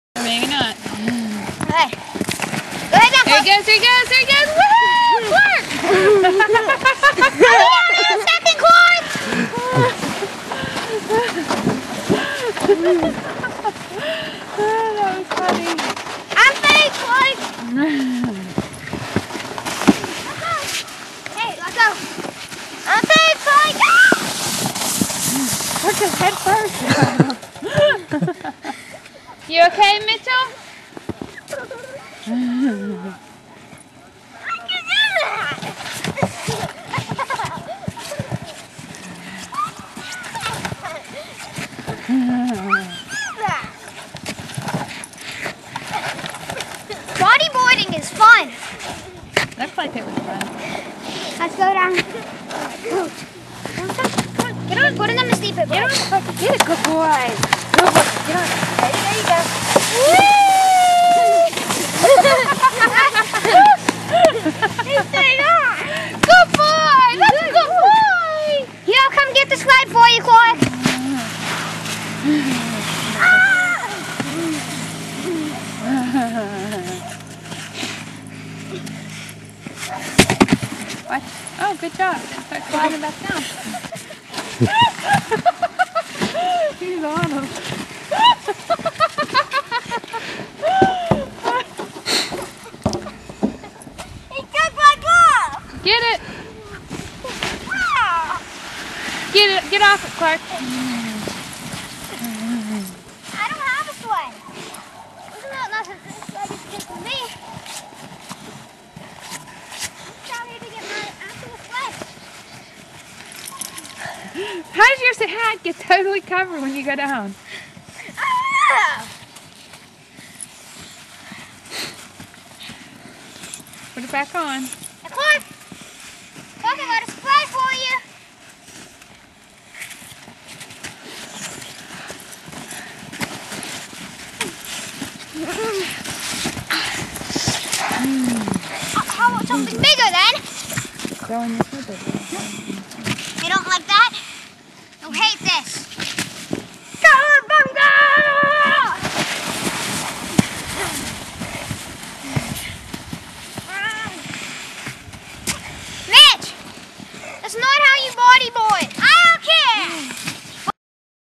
Clark The Sledriding Bulldog!
Here's our bulldog, Clark Griswold. For some reason, he really loves the snow. Watch him go sled riding and maul my kids a little! It's hard to believe that he is only around seven months old in this video. He's going to be massive!